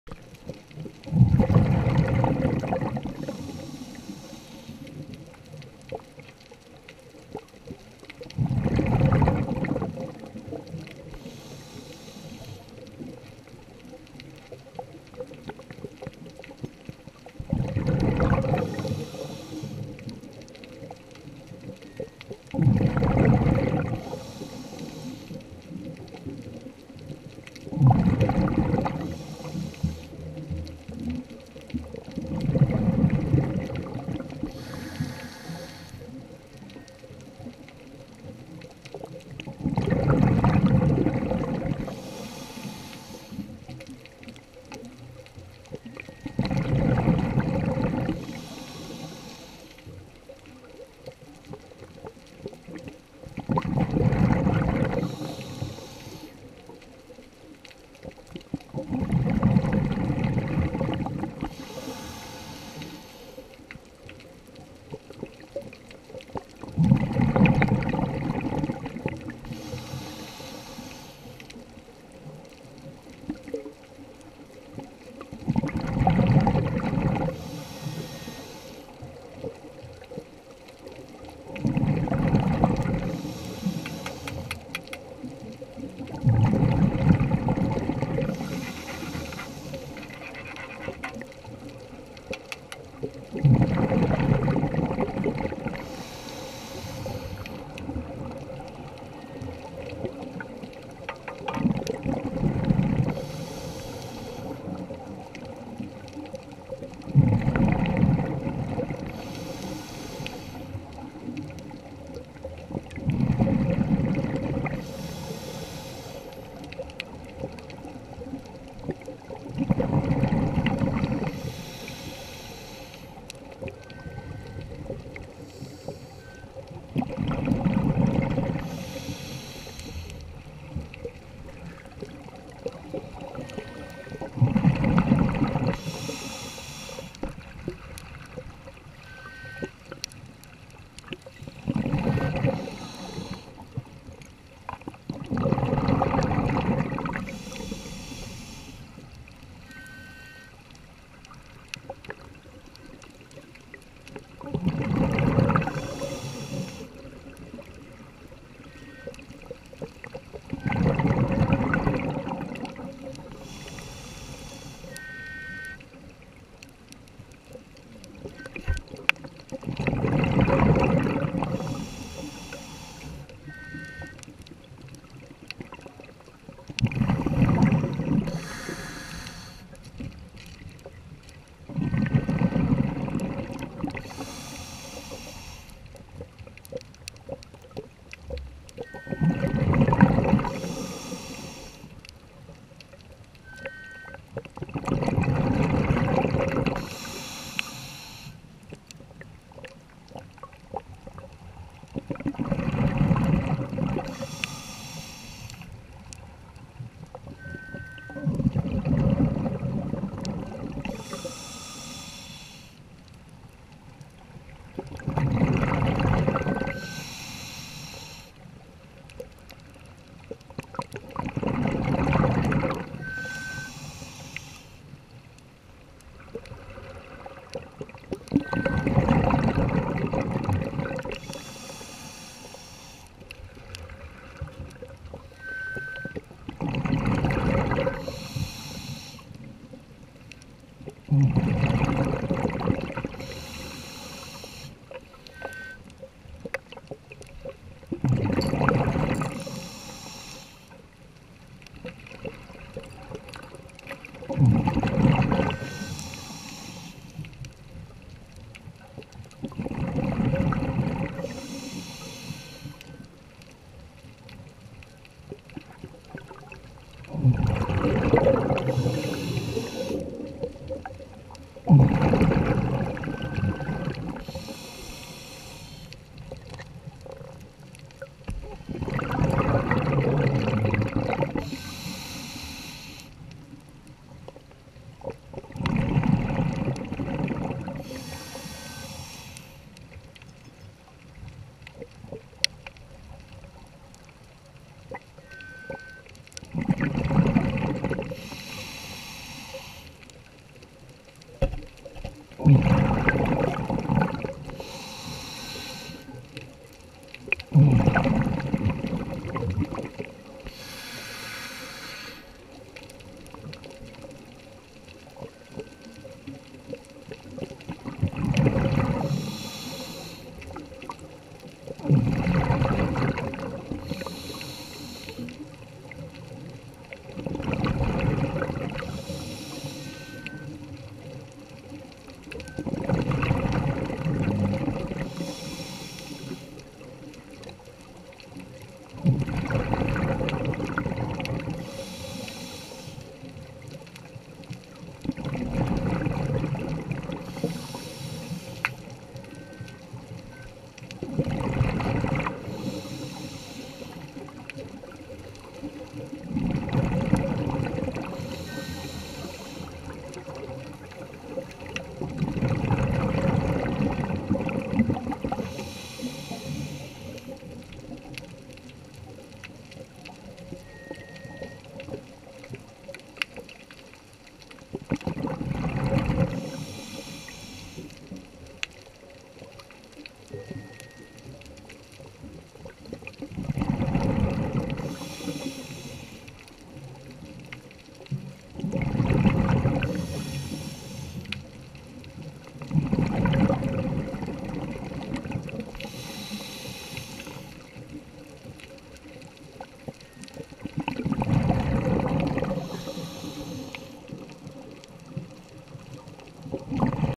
Pasikuda Wreck 2430
PASIKUDA WRECKS
Depth: 6 - 12 Meters, Location: West of Pasikuda.  Type: Steam Ships. Sunk: Late 19th century to early 20th Century. Beautiful wrecks, but visibility can be very bad. Good for new and experienced recreational divers.
References:
Dharshana Jayawardena (2016) Ghosts of the Deep - Diving the shipwrecks of Sri Lanka, Vijitha Yapa Publications, Colombo, pp.198-205.
Date 07/05/2018